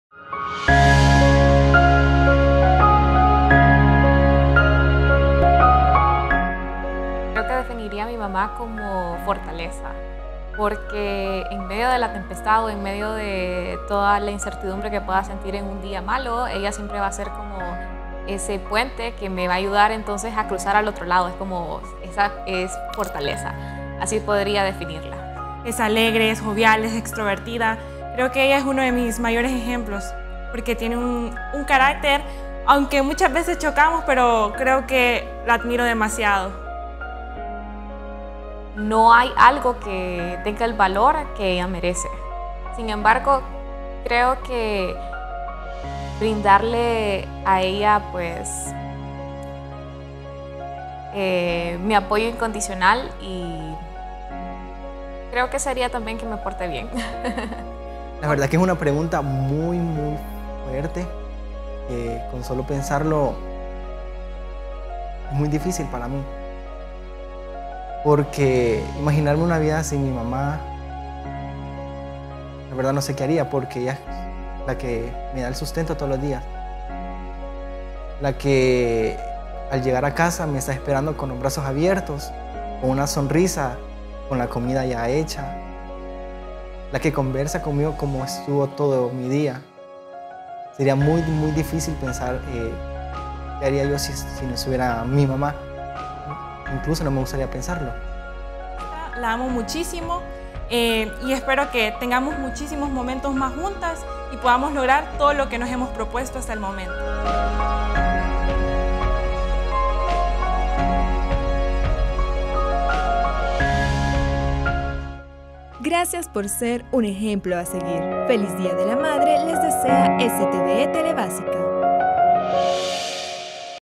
¡Feliz día de las madres!
¡Que la distancia no sea una barrera para expresarle a mamá lo que sentimos!

Desde STVE Telebásica, queremos desearle un ¡Feliz día de las madres!